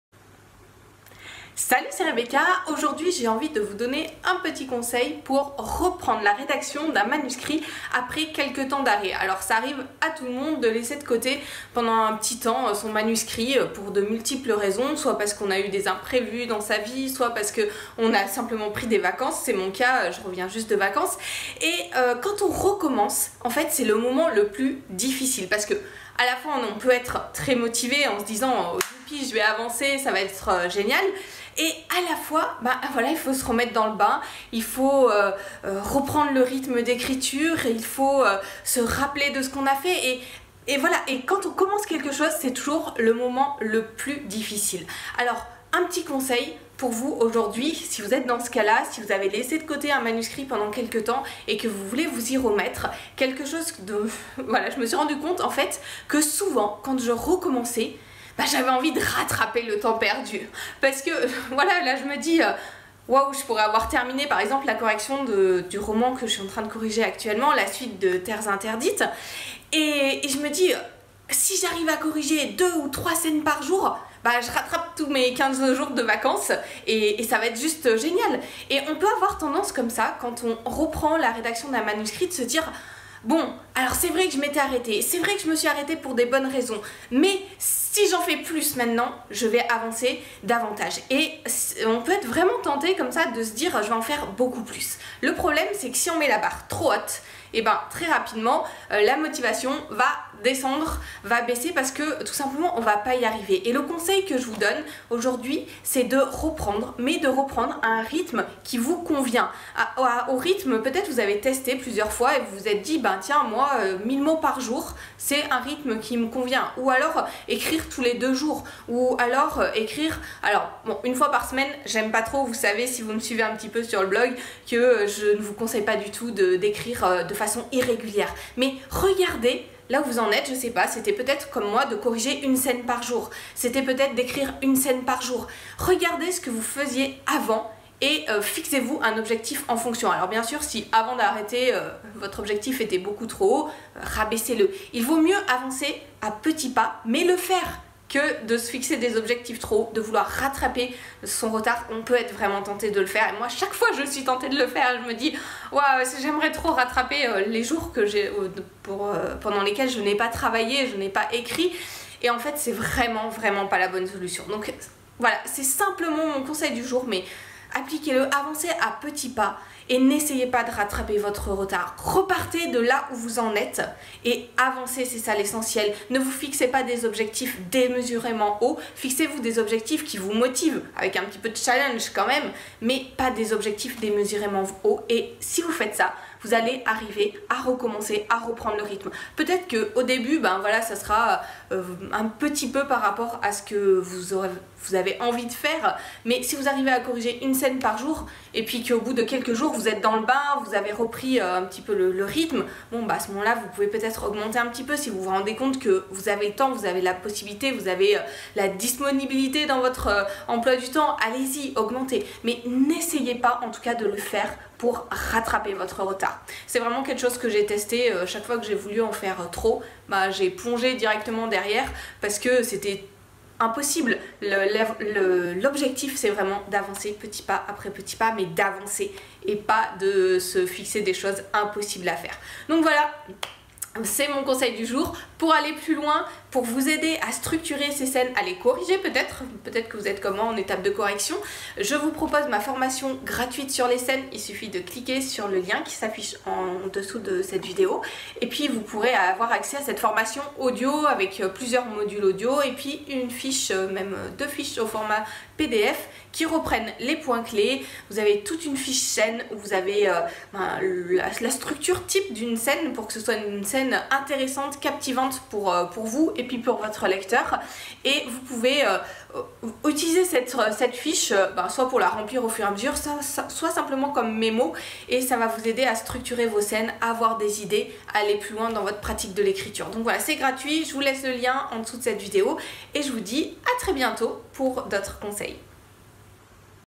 Un conseil pour recommencer à écrire
Reprendre un manuscrit après quelques jours ou semaines de pause n'est pas évident.
Découvrez ici un conseil pour vous remettre au travail et avancer sereinement votre projet d'écriture.

-Retrouvez tous les conseils pour écrire votre roman sur mon blog

-Pour recevoir un mail par jour plein de conseils pour écrire, c'est